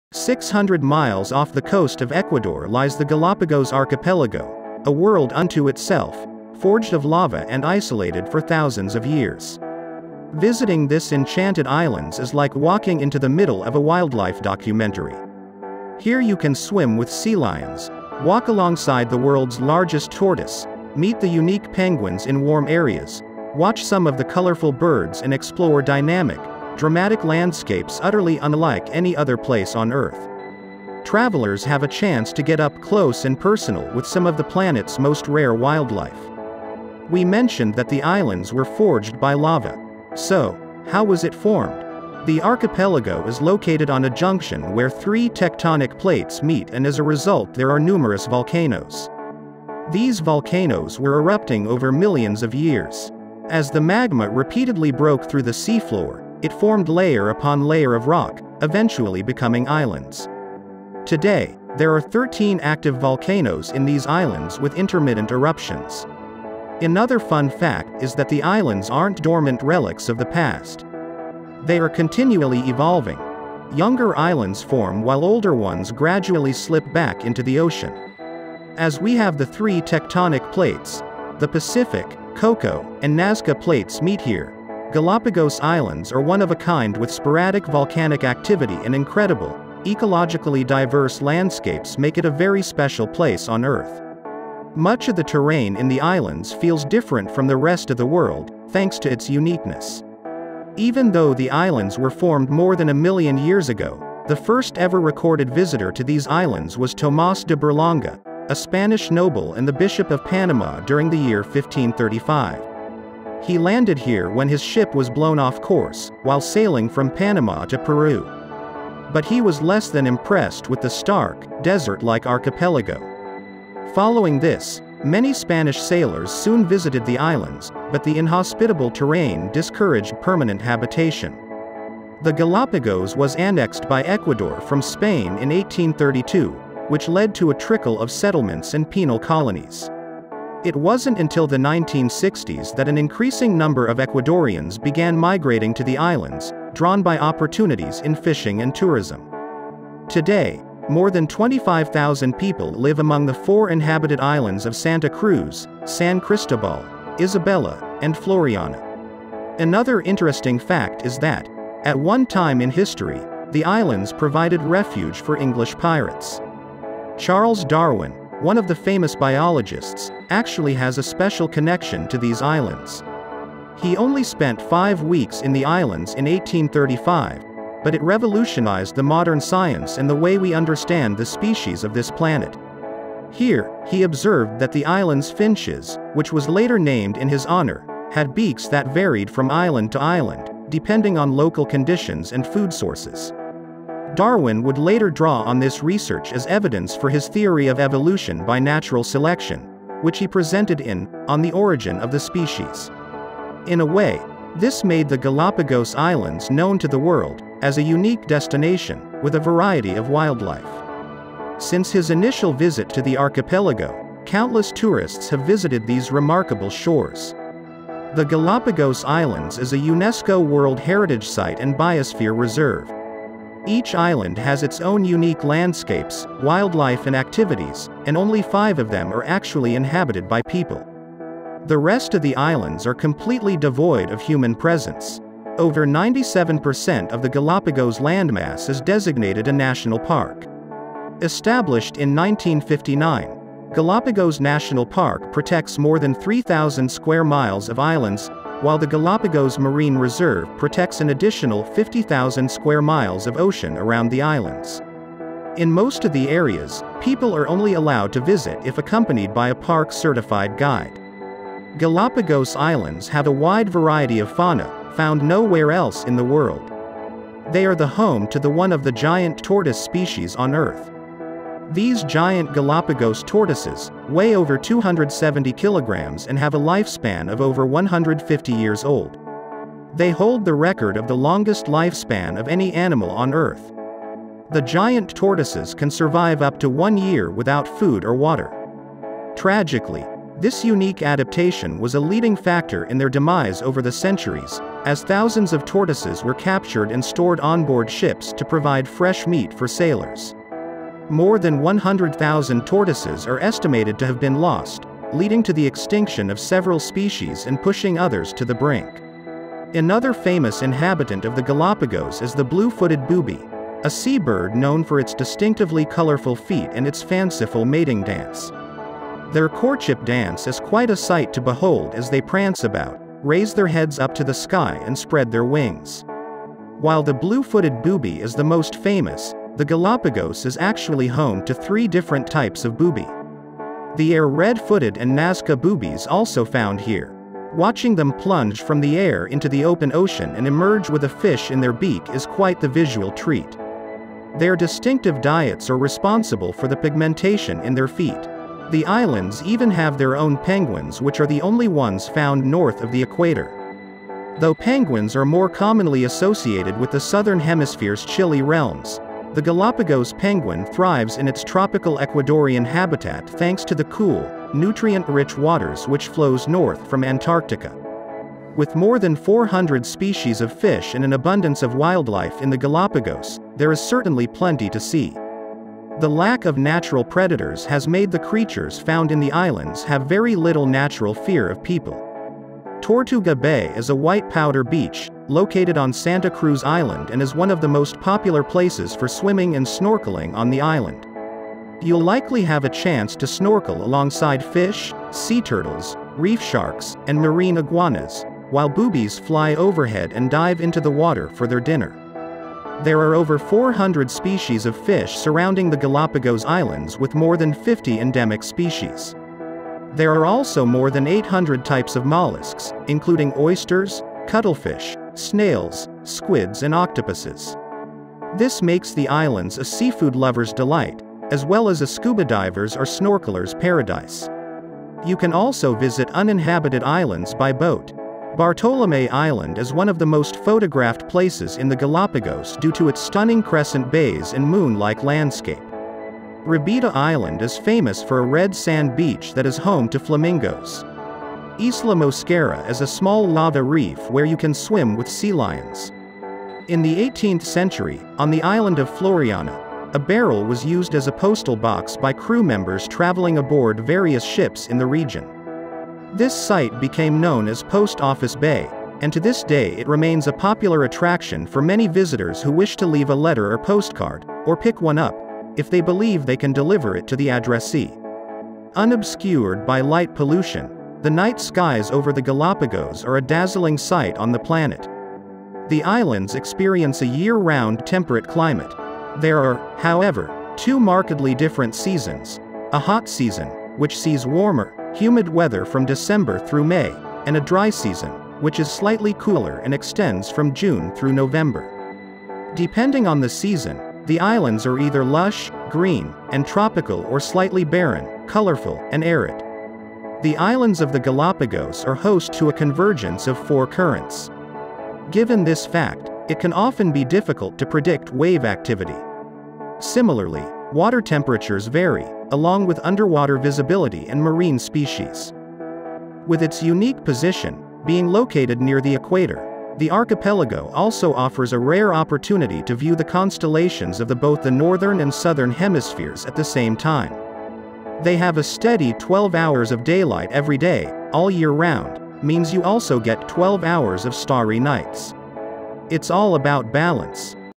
Amazing Galapagos Islands | Penguins, Giant Tortoises, Blue footed booby & a Darwin Connection
Galapagos Islands lies near the coast of Ecuador. With its amazing beauty and incredible wildlife such as Penguins, Giant tortoises and the blue footed, it is no less than a paradise for a nature lover. This island has a very important place in science due to Charles Darwin's research on 'The theory of Evolution'.

Visiting this Enchanted Islands is like walking into the middle of a wildlife documentary. Here you can swim with sea lions, walk alongside the world’s largest tortoise, meet the unique penguins in warm areas, watch some of the colourful birds and explore dynamic, dramatic landscapes utterly unlike any other place on Earth. Travellers have a chance to get up close and personal with some of the planet’s most rare wildlife.

Charles Darwin, one of the famous biologists, actually has a special connection to these islands. He only spent five weeks in the islands in 1835, but it revolutionized the modern science and the way we understand the species of this planet.  Here, he observed that the islands’ finches, which was later named in his honour, had beaks that varied from island to island, depending on local conditions and food sources. Darwin would later draw on this research as evidence for his theory of evolution by natural selection, which he presented in ‘On the Origin of the Species’.

The Galapagos Islands is a UNESCO World Heritage Site and Biosphere Reserve. Each island has its own unique landscapes, wildlife and activities, and only five of them are actually inhabited by people. The rest of the islands are completely devoid of human presence. Over 97 percent of the Galápagos landmass is designated a national park. Established in 1959, Galápagos National Park protects more than 3,000 square miles of islands, while the Galápagos Marine Reserve protects an additional 50,000 square miles of ocean around the islands. In most of the areas, people are only allowed to visit if accompanied by a park-certified guide.

Galapagos islands have a wide variety of fauna, found no where else in the world. They are the home to the one of the giant tortoise species on earth. These Giant Galapagos Tortoises, weigh over 270kg and live over 150 years old. They hold the record of the longest life span of any animal on Earth! The giant tortoises can survive up to one year without food or water. Tragically, this unique adaptation was a leading factor in their demise over the centuries, as thousands of tortoises were captured and stored onboard ships to provide fresh meat for sailors. More than 100,000 tortoises are estimated to have been lost, leading to the extinction of several species and pushing others to the brink.

Another famous inhabitant of the Galápagos is the blue-footed booby, a sea bird known for its distinctively colourful feet and its fanciful mating dance. Their courtship dance is quite a sight to behold as they prance about, raise their heads up to the sky and spread their wings. While the blue-footed booby is the most famous, the Galapagos is actually home to three different types of booby.

The islands even have their own penguins which are the only ones found north of the Equator. Though penguins are more commonly associated with the Southern Hemisphere’s chilly realms, the Galápagos penguin thrives in its tropical Ecuadorian habitat thanks to the cool, nutrient-rich waters which flows north from Antarctica. The lack of natural predators has made the creatures found in the islands have very little natural fear of people.

Tortuga Bay is a white powder beach, located on Santa Cruz Island and is one of the most popular places for swimming and snorkelling on the island. You’ll likely have a chance to snorkel alongside fish, sea turtles, reef sharks, and marine iguanas, while boobies fly overhead and dive into the water for their dinner.

You can also visit uninhabited islands by boat. Bartolome island is one of the most photographed places in the Galapagos due to its stunning crescent bays and moon-like landscape. Rabida island is famous for a red sand beach that is home to flamingos. Isla Mosquera is a small lava reef where you can swim with sea lions.

Unobscured by light pollution, the night skies over the Galápagos are a dazzling sight on the planet. The islands experience a year-round temperate climate. There are, however, two markedly different seasons: a hot season, which sees warmer, humid weather from December through May, and a dry season, which is slightly cooler and extends from June through November. Depending on the season, the islands are either lush, green, and tropical or slightly barren, colourful, and arid.

With its unique position, being located near the Equator, the archipelago also offers a rare opportunity to view the constellations of the both the Northern and Southern Hemispheres at the same time. They have a steady 12 hours of daylight every day, all year round, means you also get 12 hours of starry nights. It’s all about balance!